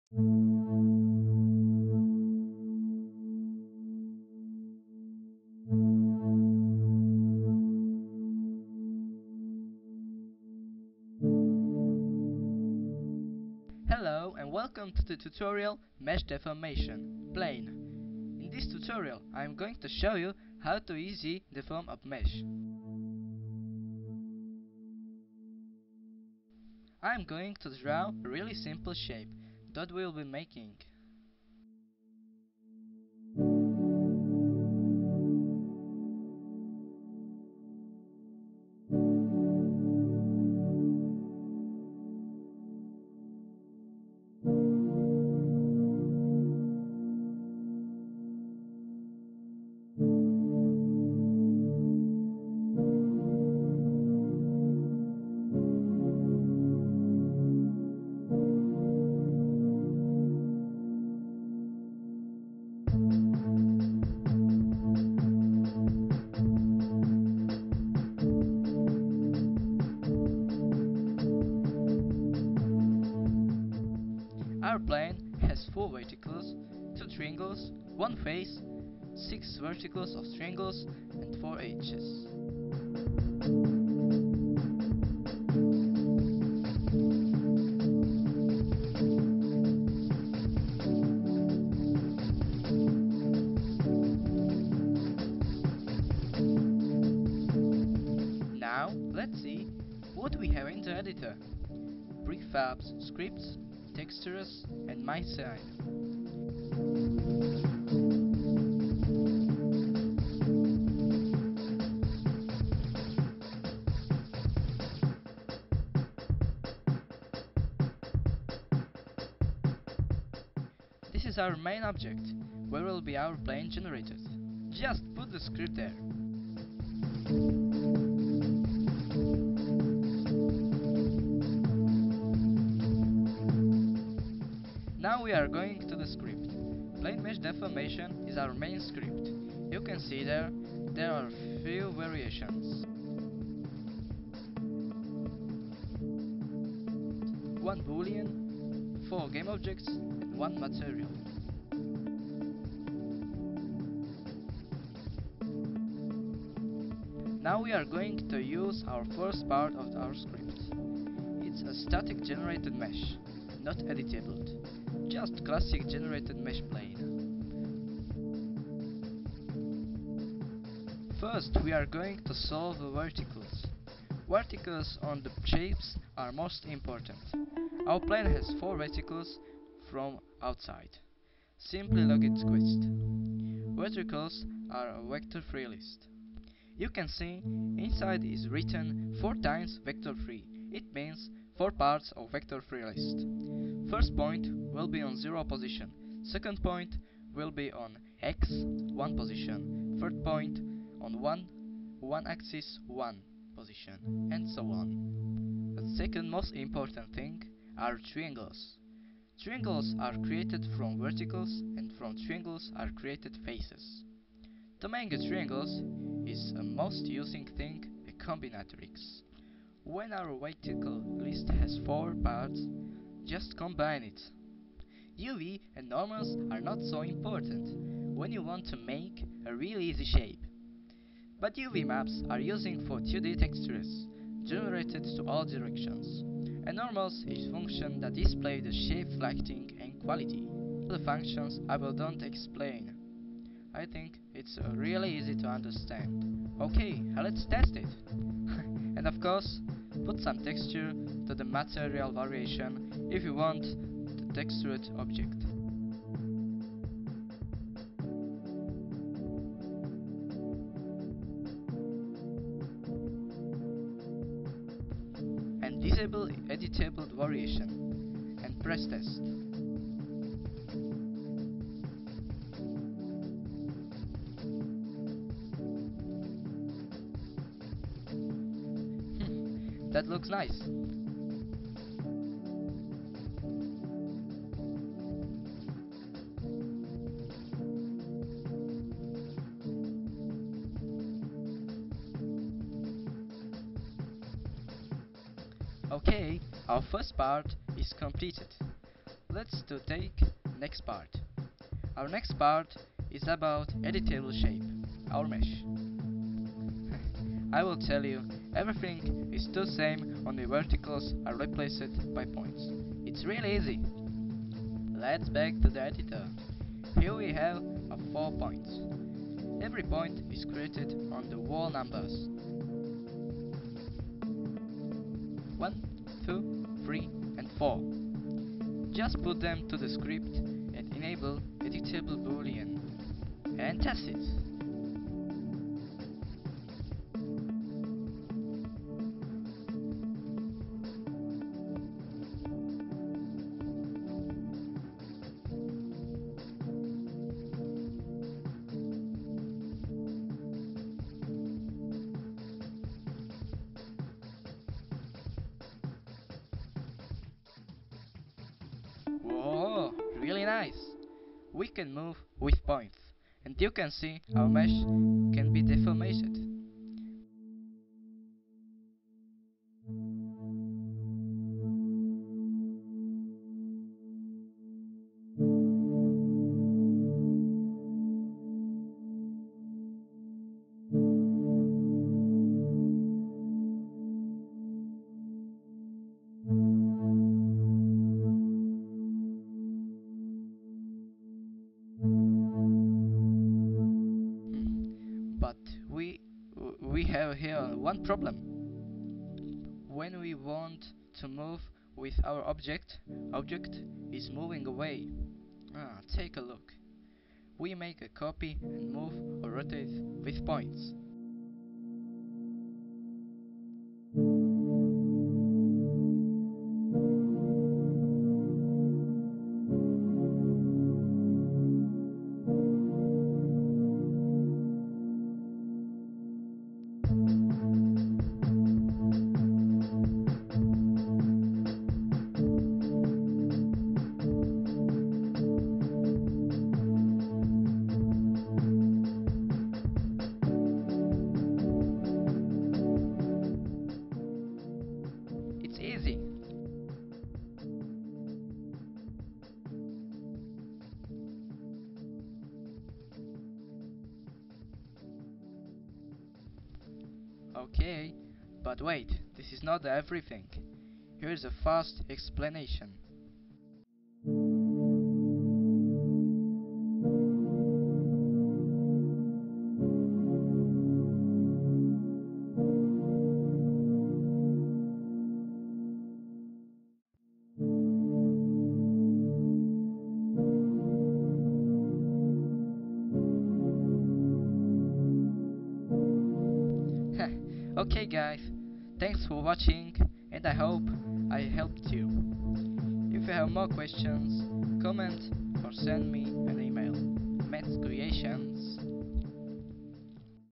Unity 3D - Mesh Deformation Tutorial (Plane) #1 Basics
This is my first tutorial that I've ever made, and it's also the first video tutorial of mesh deformation sequel. Sorry for my horrible pronunciation! I was just 14 that time!
Update 2021: My first and probably the last video tutorial lol!